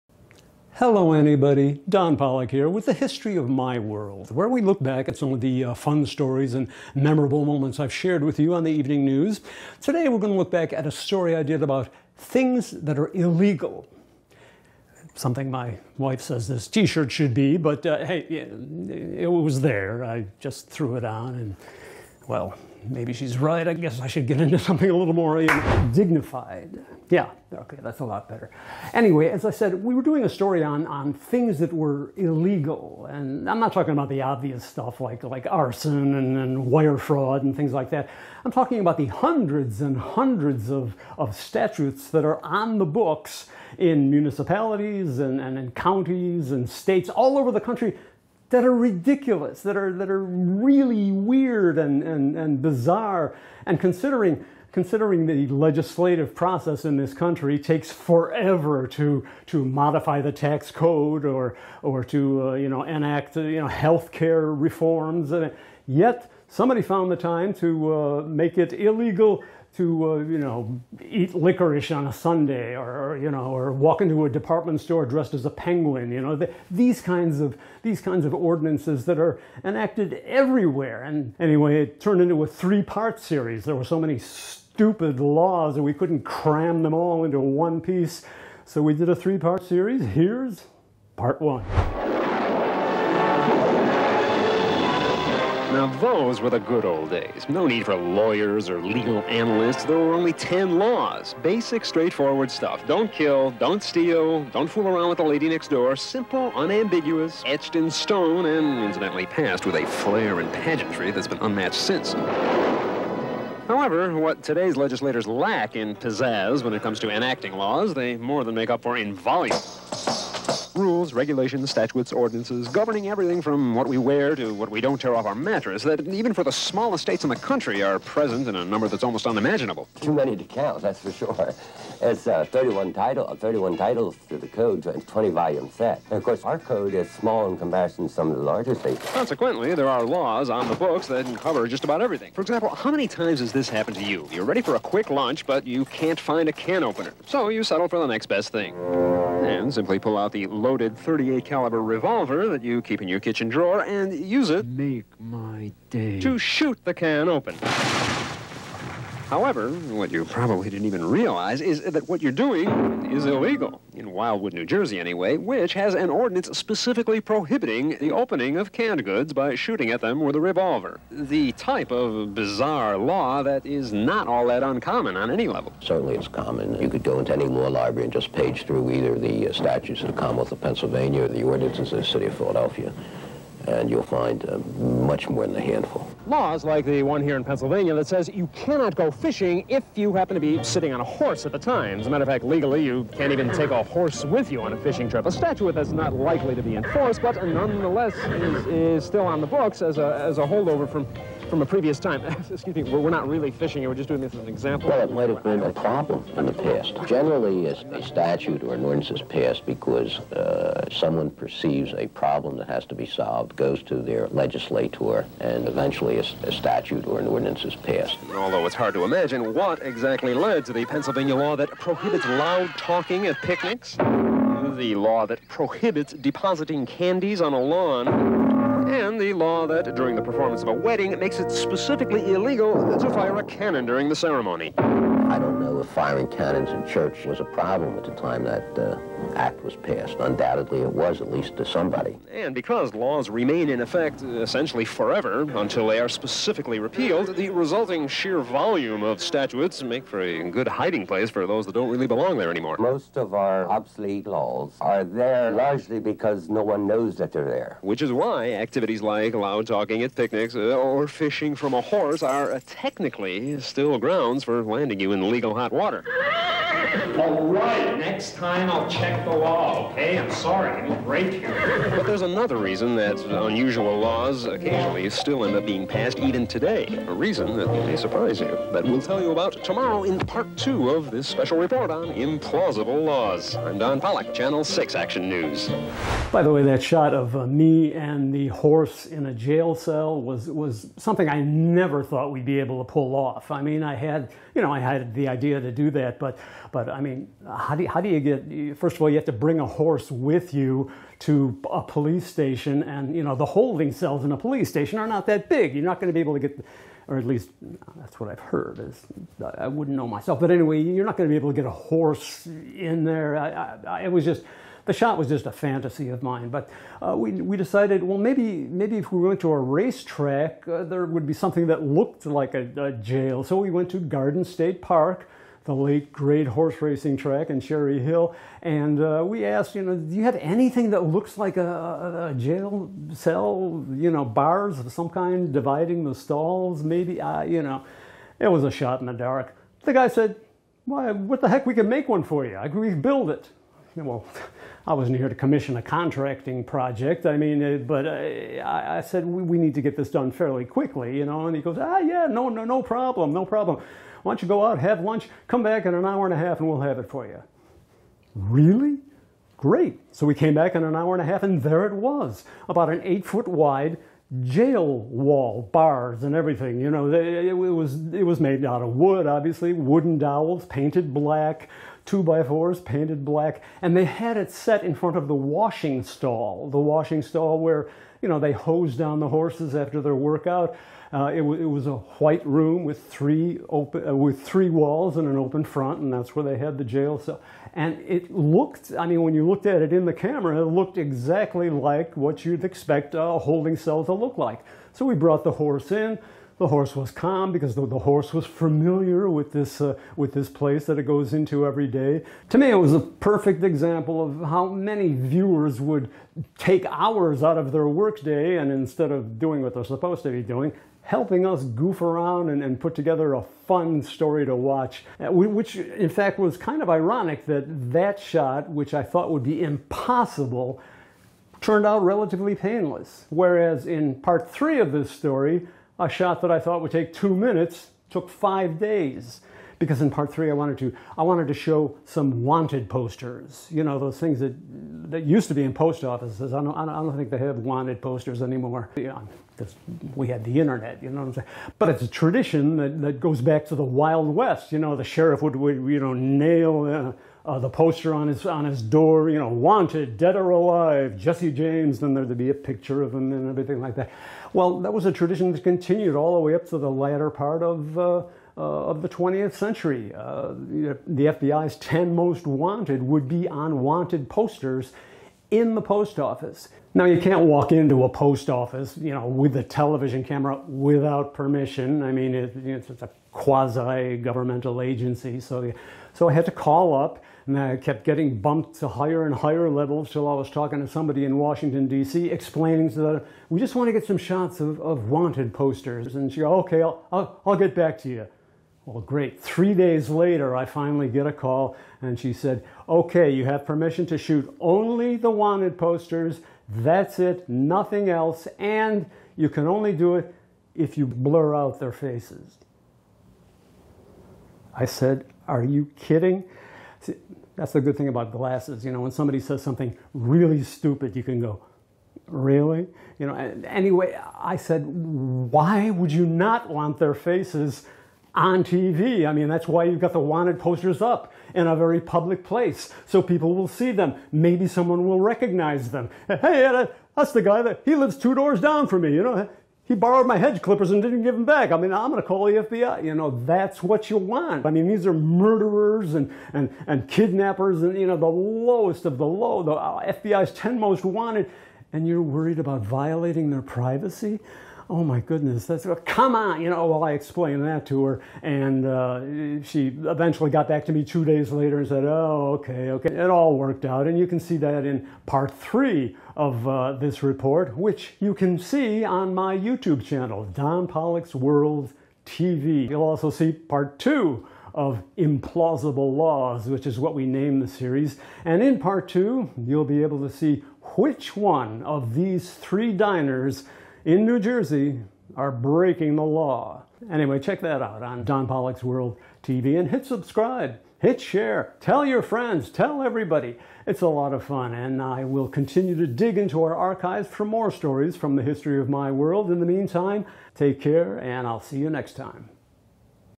You may be a felon... and not even know it!
Don Polec takes a closer look at some the more bizarre, weird and ridiculous laws that are still on the books in municipalities and states across the country.